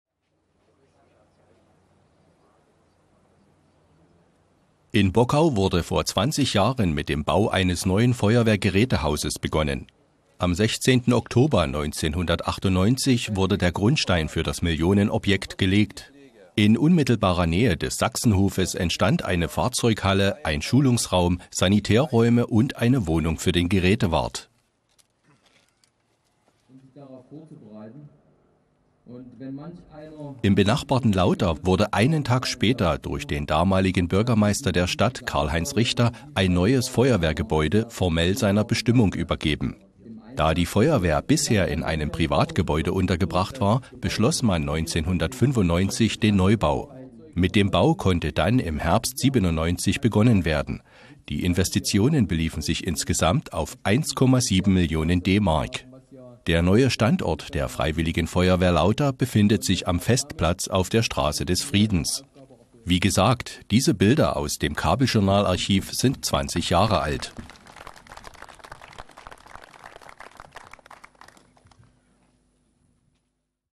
Damals: Neue Feuerwehrgebäude
BOCKAU/LAUTER-BERNSBACH: In Bockau wurde vor 20 Jahren mit dem Bau eines neuen Feuerwehrgerätehauses begonnen (Bild).